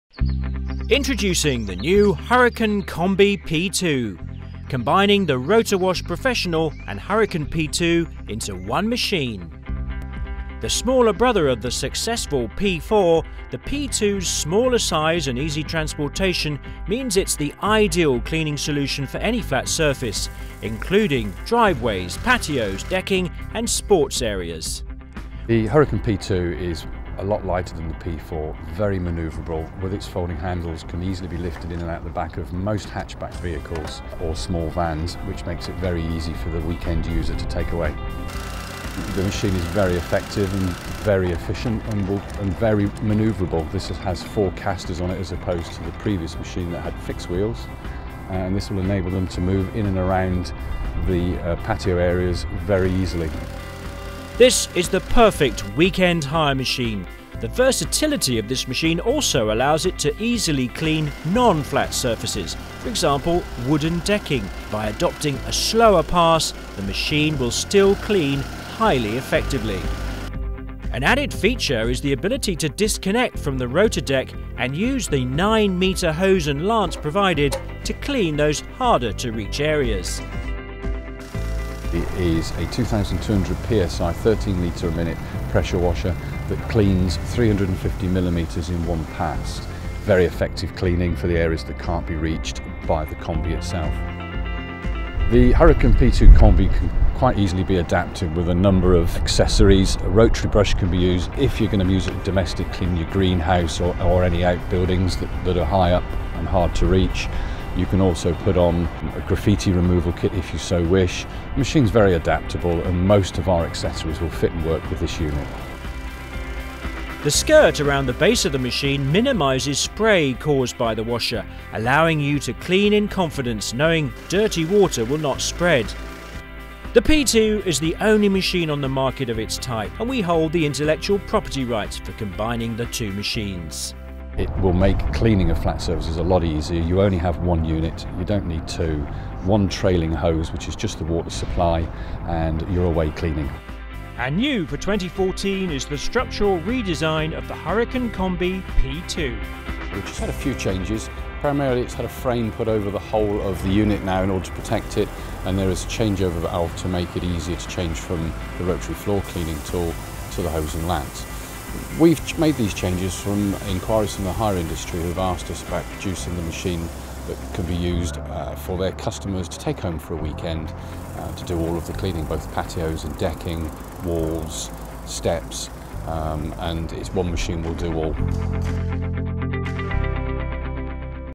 Wellers Hire - Combi Petrol Pressure Washer
It combines the Rotowash Professional and Hurricane P2 pressure washer into one machine. Making it easy to transport and the ideal cleaning solution for patios, decking and smaller jobs.

This is the perfect ‘weekend’ hire machine for Hire Companies nationwide. It has the added bonus of being able to disconnect from the rotodeck and use with a 9m hose and lance provided to clean harder to reach areas.

The P2 Combi is 450mm wide with a 300mm cleaning width and comes complete with hose and lance and the option of either a fixed or folding handle. The Combi will draw water from 7m ½” hose and 11m ¾”.